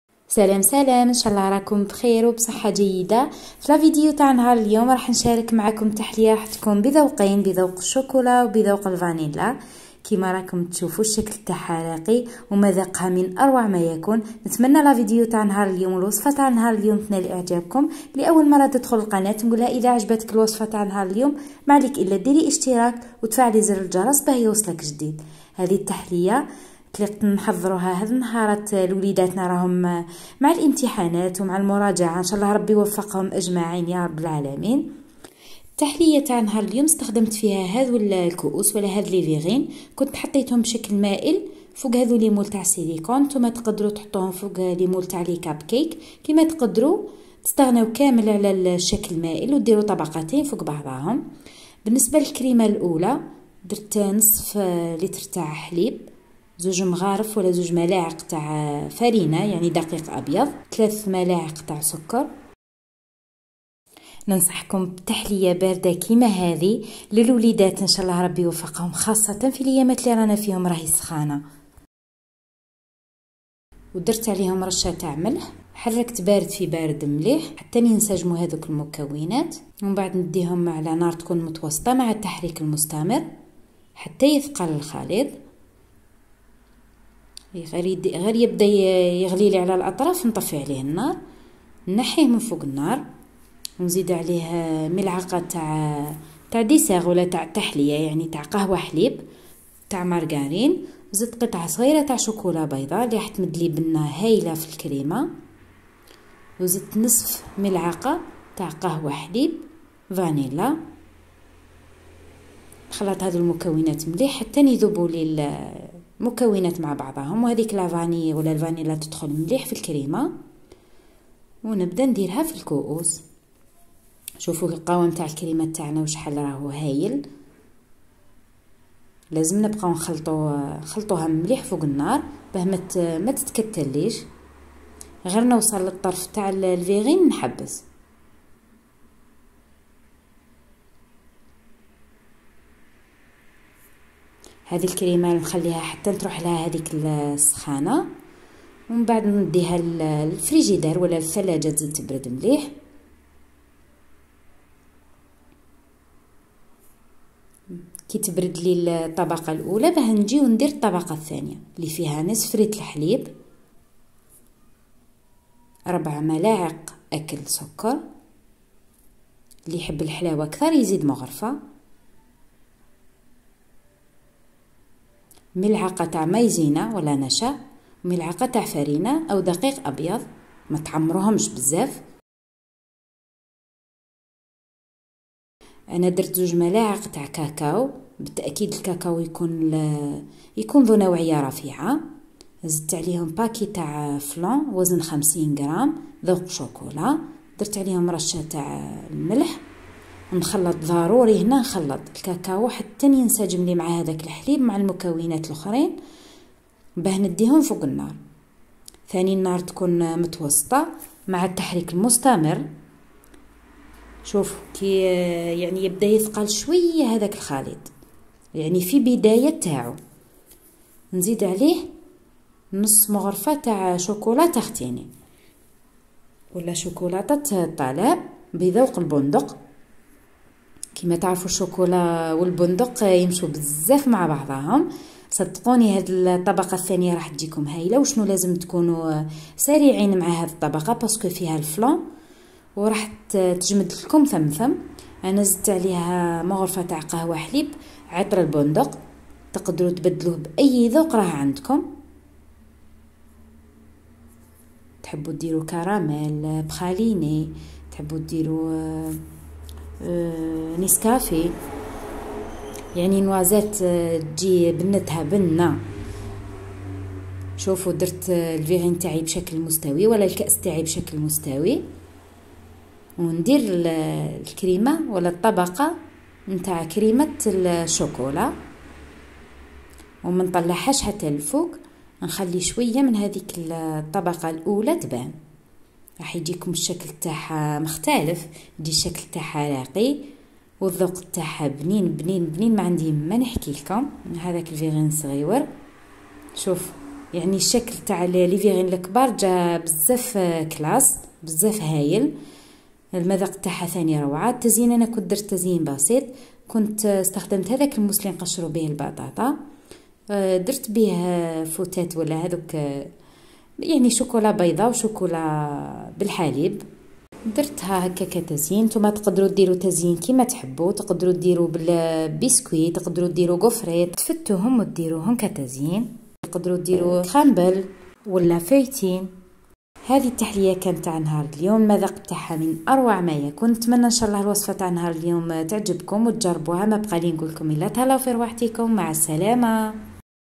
فالسخانة ☀️🌡️تليق تحلية باردة تبرد القلب 🤤لكل من يعشق الشوكولا 🍫🌰 باردة وبنييينة 🤩
تحلية تؤكل باردة وبرودتها حنينة متمرضوش باذن الله وتبرد القلب
مقادير الطبقة الاولى
نصف لتر حليب
ملعقتين فرينة(دقيق أبيض)
3ملاعق سكر
رشة ملح
قطعة صغيرة شوكولا بيضاء
ملعقة تحلية مارغرين
نصف ملعقة تحلية فانيلا
مقادير الطبقة الثانية
نصف لتر حليب
ملعقة غير مملوءة مايزينة(نشأ)
ملعقة غير مملوءة فرينة (دقيق ابيض)
رشة ملح
ملعقتين كاكاو بني نوعية جيدة
علبة فلان ذوق شوكولا
نصف ملعقة شوكولا للطلاء شوق بندق
عطر بندق (او اي عطر تختاروه حسب الرغبة)

التزيين( حسب الرغبة)